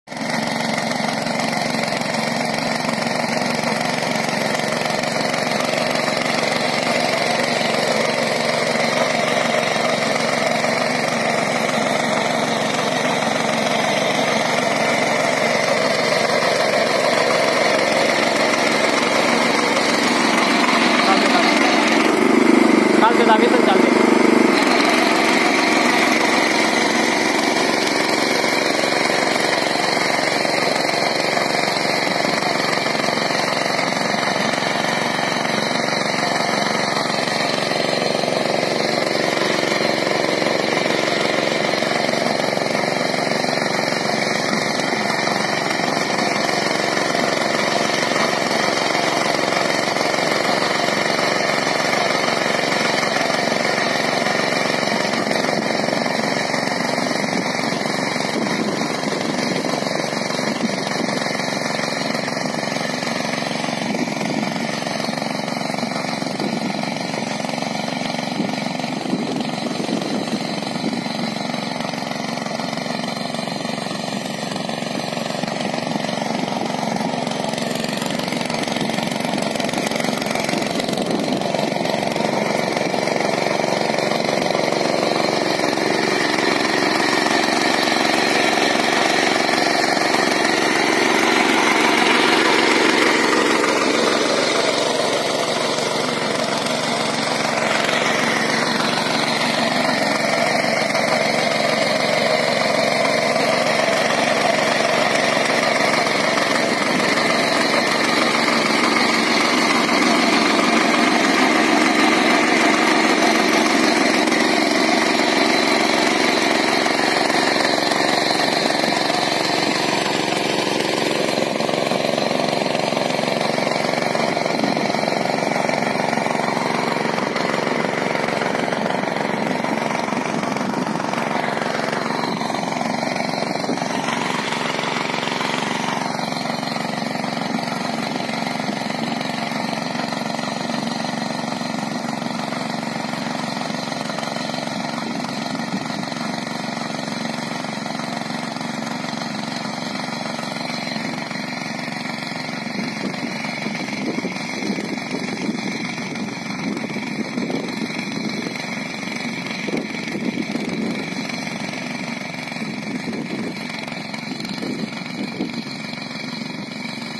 7 hp पावर विडर 🍅🍅🍅 🍉 🍉 🍉
7 एच पी पावर विडर या मशीन ने पिकांमधील अंतरमशागत करता येते मो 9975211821 farmequipment agriculture shetkri @shriagromachineries